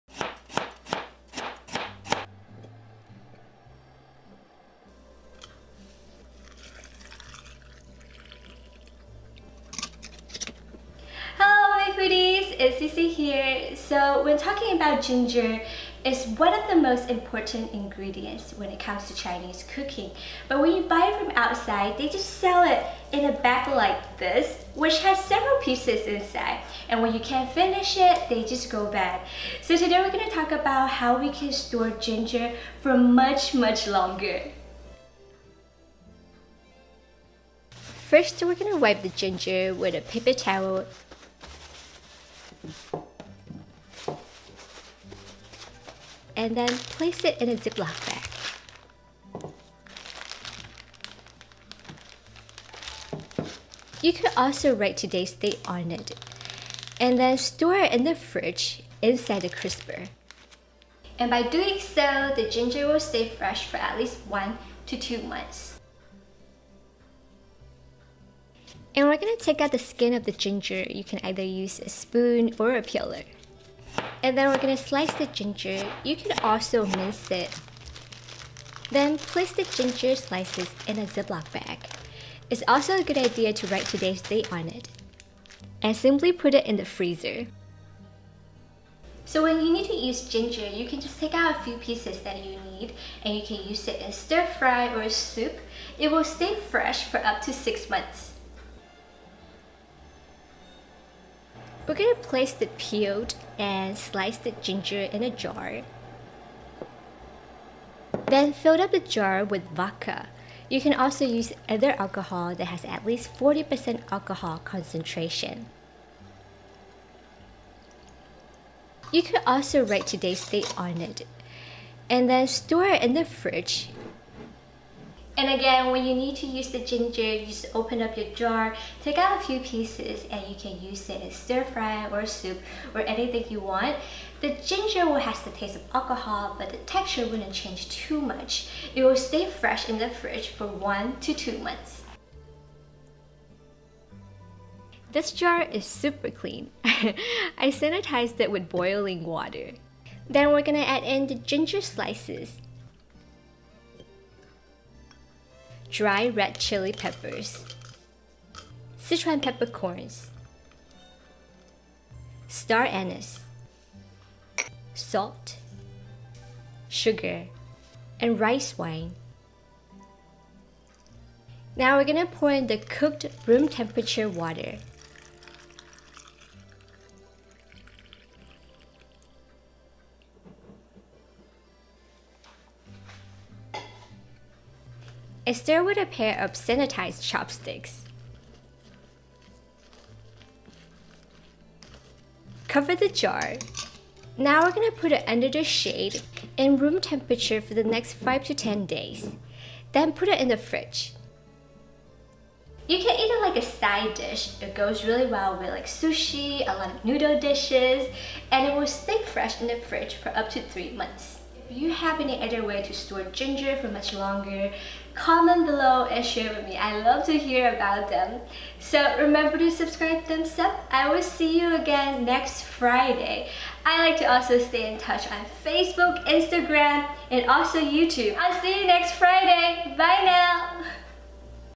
4 Ways to Store Ginger for Months, Awesome Kitchen Tips! CiCi Li - Asian Home Cooking Recipes
Ginger is one of the most important ingredients for Chinese cooking. They sometimes sell it in a bag, which has many pieces inside. So what are the best ways the store ginger?

1. Store whole ginger in a Ziploc bag in the fridge’s crisper drawer.
First, wipe the ginger with a paper towel. Then place it in a Ziploc bag. You could also today’s date on it. Store in the fridge, inside the crisper. This will stay fresh for 1 to 2 months.

2. Freeze peeled, sliced, or minced ginger in the freezer.
First, peel, and slice the ginger (you can also mince it). Place the ginger slices in a Ziploc bag. It’s also good to write today’s date on it. When you use it, just take the out the pieces that you need, and use them in stir-fry or soup. It will stay fresh for up to 6 months.

3. Store peeled, sliced ginger in a jar of vodka in the fridge.
Place peel and sliced ginger in a jar. Then pour vodka in it to fill everything up. (You can also use other alcohol that has at least 40% alcohol concentration.) When you use it, you can just take out the portion that you need and use it in stir-fry or soup. The ginger will contain the taste of alcohol, but the texture will stay nice and firm. It will stay fresh for 1 to 2 months.

4. Make pickled ginger.

Ingredients:
6 ounces ginger, peeled, sliced
1 tablespoon dried red chili peppers, washed with cooked water
1 tablespoon Sichuan peppercorns, washed with cooked water
1 star anise, washed with cooked water
1/3 teaspoon salt, no iodize
1 teaspoon sugar
2 tablespoons rice wine
1 cup water, cooked, room temperature

In a jar (I’ve already sanitized with boiling water.), add in the ginger slices, dried red chili peppers, Sichuan peppercorns, star anise, salt, sugar, and rice wine. Pour in the cooked room temperature water. Stir with a pair of sanitized chopsticks. Cover the jar.
Put it, under the shade, under room temperature for the next 5 to 10 days. Then put it in the fridge. You can eat it like a side dish. It will stay fresh for up to 3 months.